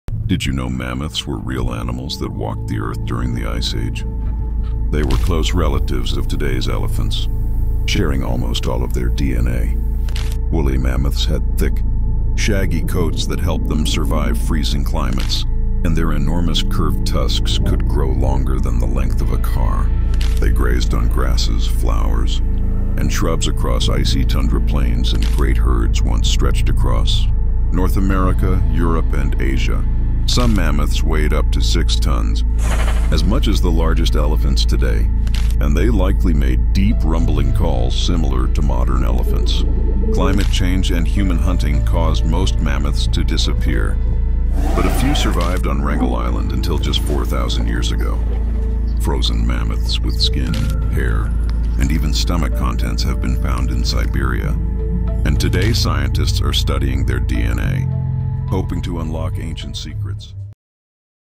Ice age Giants 🦣
Long before humans built cities, giant woolly mammoths walked the frozen lands of the Ice Age. With thick fur, massive tusks, and incredible strength, they braved the cold where few could survive. Discover how these ancient giants lived, fought, and ultimately disappeared - leaving behind secrets still frozen in time.

If mammoths were alive today, could they survive in our changing world?🌏